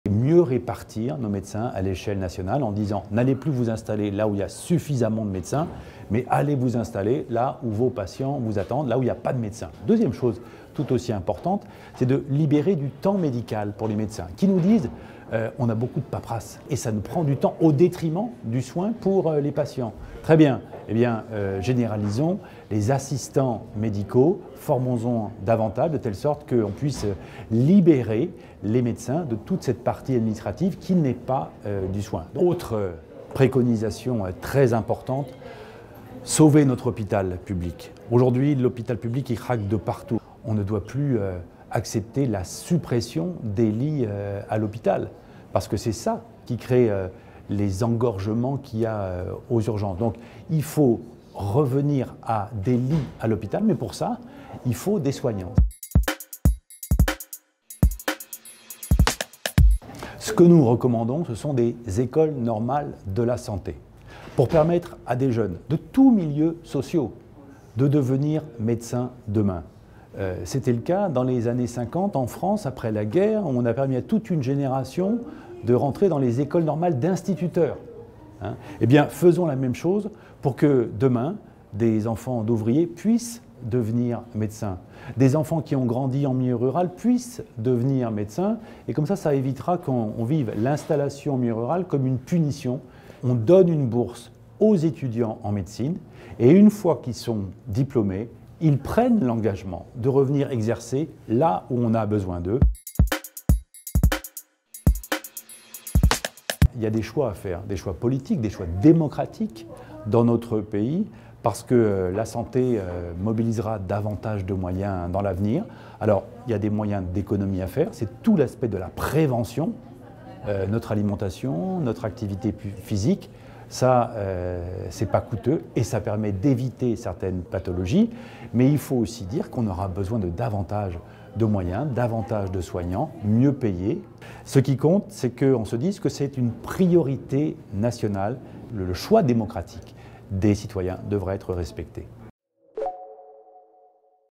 Comment améliorer l’accès aux soins ?
Alors que le vieillissement de la population et la progression des maladies chroniques accroissent les besoins en soins, à l’heure où le doublement des franchises médicales, affichées comme un élément de responsabilisation, peuvent entrainer des renoncements aux soins et que la désertification médicale s’accentue en France : comment donner un meilleur accès à la santé à tous les Français ?

Invité du Séminaire des mandatés locaux de la Mutualité Française, Guillaume Garot, député de la Mayenne, présente ses préconisations.